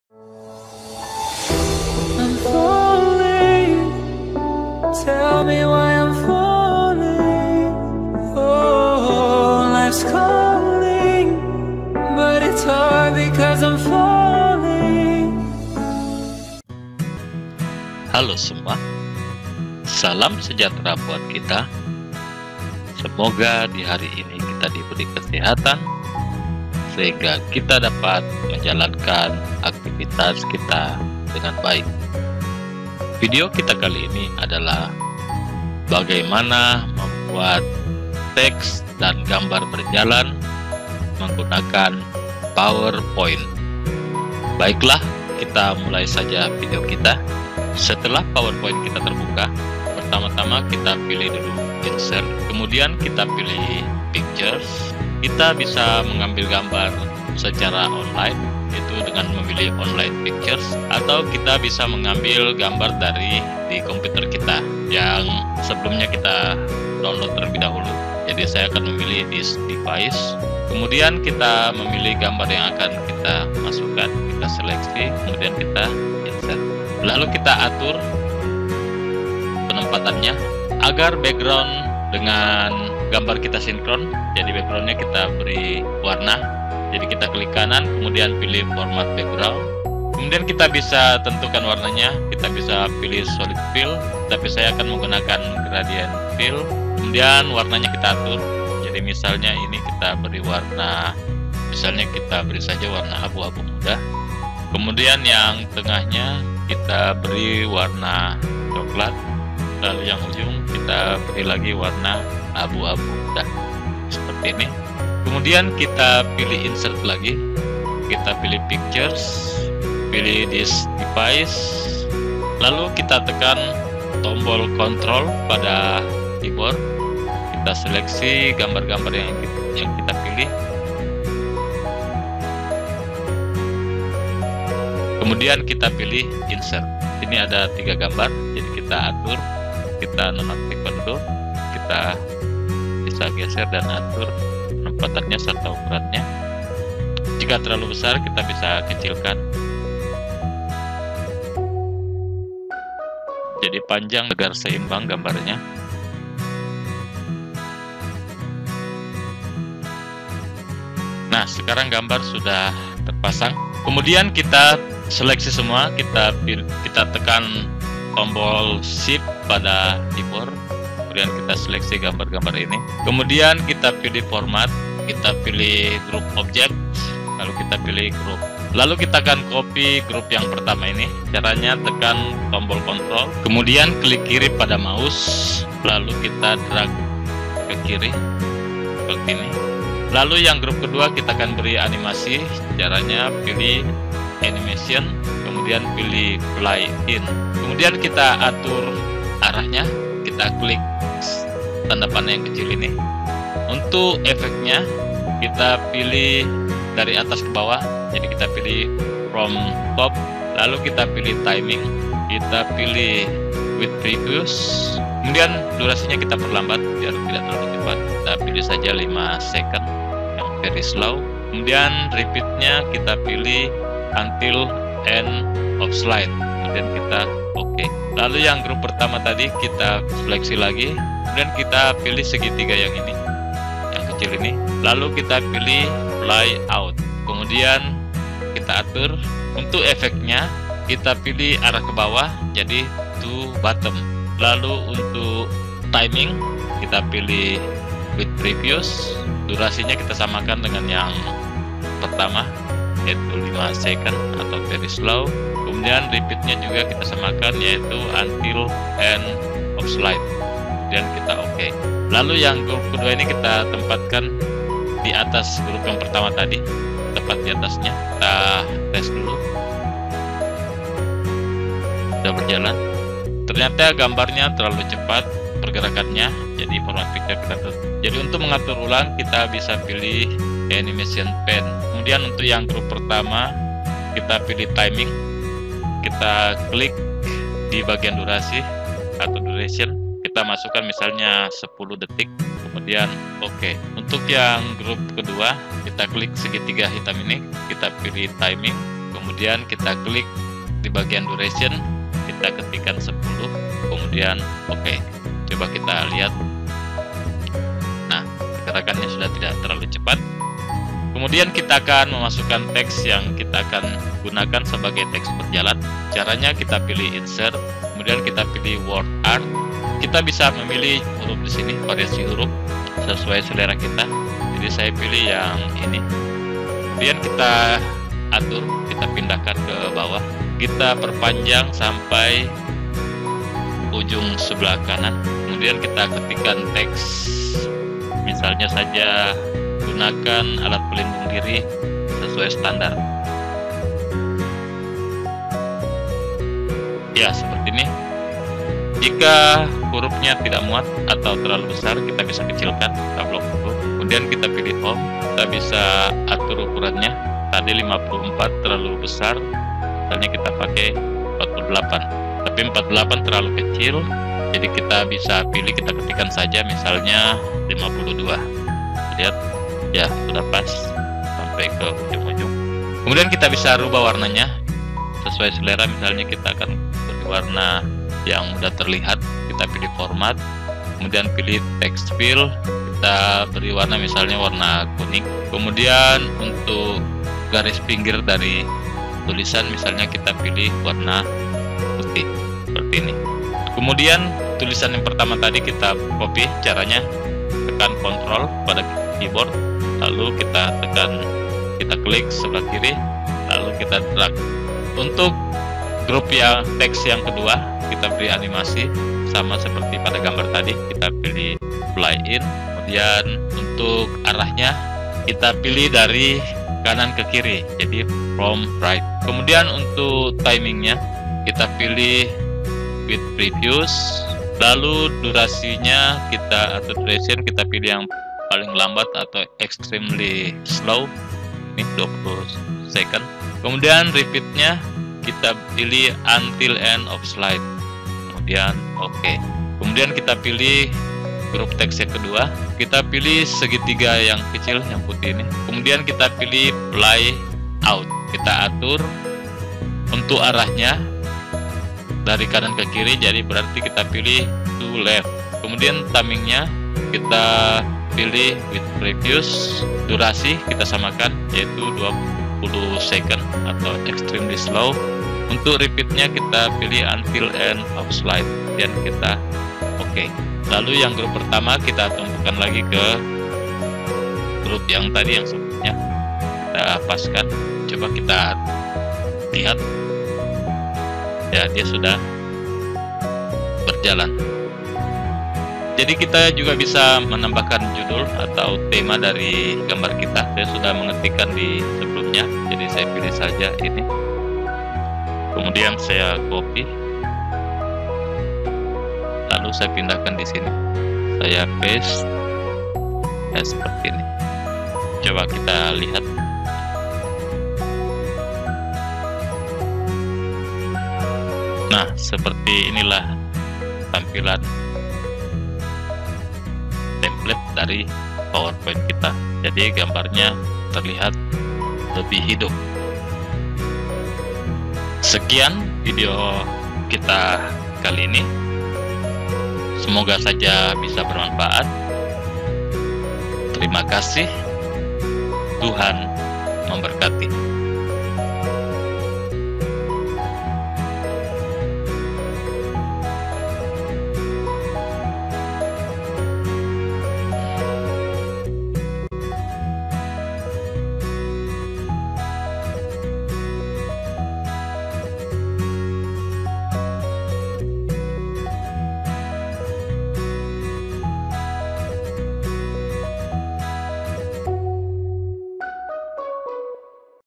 CARA MEMBUAT TEXT DAN GAMBAR BERJALAN DENGAN POWERPOINT
CARA MEMBUAT TEXT DAN GAMBAR BERJALAN DENGAN POWERPOINT. Video ini berisi tutorial bagaimana membuat text dan gambar berjalan menggunakan PowerPoint.

Atas kunjungan anda, kami ucapkan Terima Kasih.